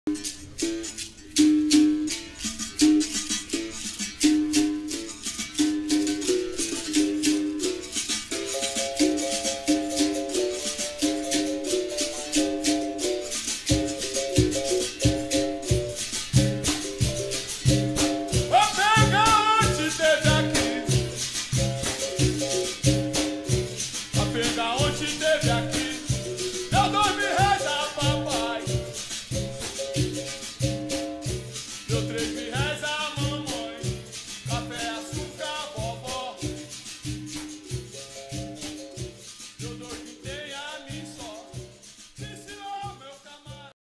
Prof. Torvelito e M. Ganso - Regional
Un jogo más con la persona q admiro tanto n este gran mundo de la Capoeira Gracias de nuevo por la oportunidad Mestre.